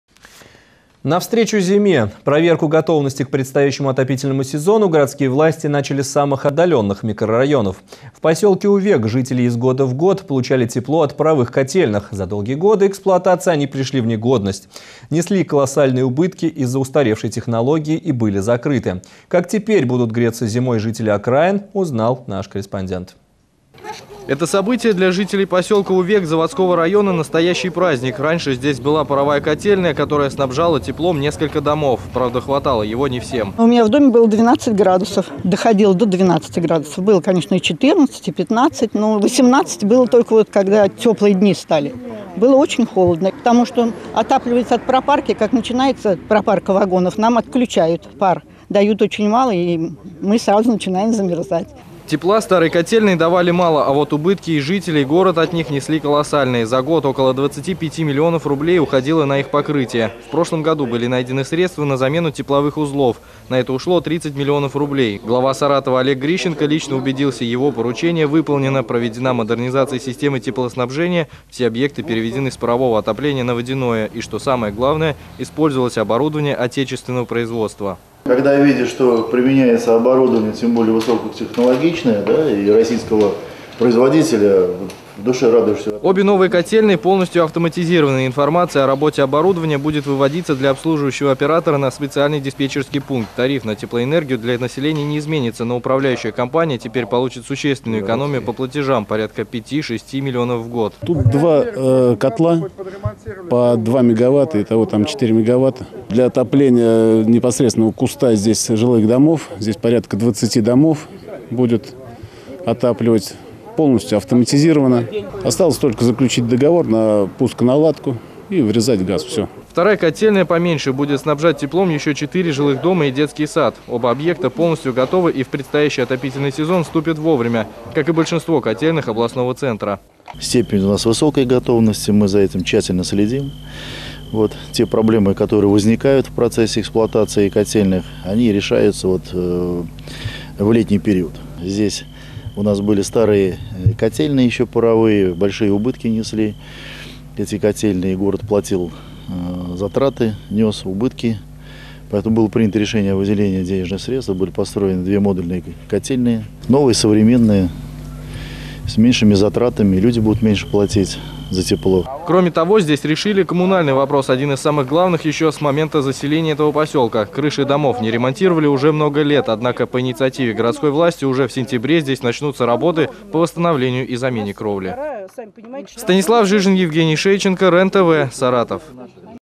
Подготовка к отопительному сезону началась с поселка Увек
Проверку готовности к предстоящему отопительному сезону городские власти начали с самых отдаленных микрорайонов. В поселке Увек жители из года в год получали тепло от паровых котельных. За долгие годы эксплуатации они пришли в негодность и были закрыты. Как теперь будут греться зимой жители окраины, узнал наш корреспондент. Сюжет вышел 2 сентября.